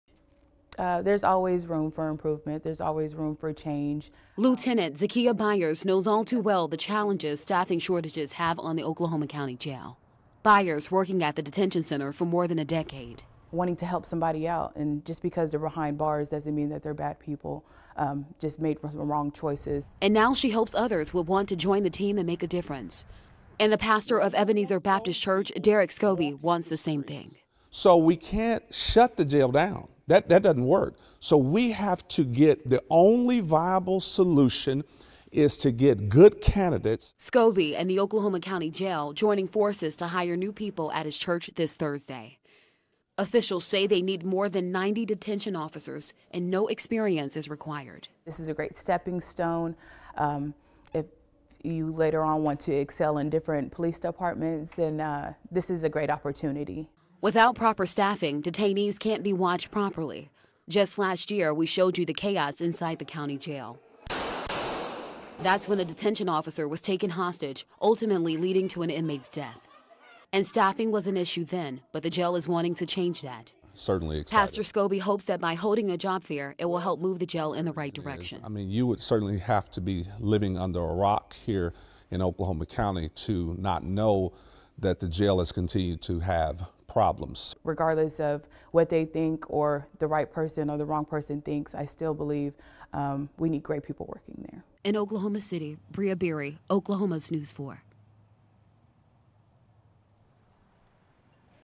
Oklahoma County Jail seeks to fill dozen of job positions
The OCCJA is joining forces with Ebenezer Church and Pastor Derrick Scobey for a job fair on Thursday.